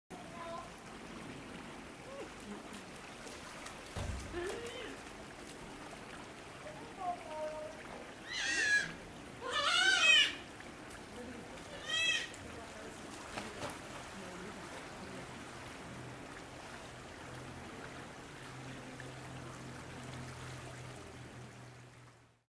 Sekiu the Sea Otter reunited after surgery
Sekiu the sea otter is doing well after her uterine mass removal last month. The 9-year-old otter can be seen swimming and foraging with pals Libby and Moea in the Rocky Shores habitat. Our Head Veterinarian Dr. Karen Wolf said, “This type of tumor rarely spreads to other organs so her surgery should be curative. We will continue to check for spreading via x-rays during her routine exams, but we’re very happy about the results.”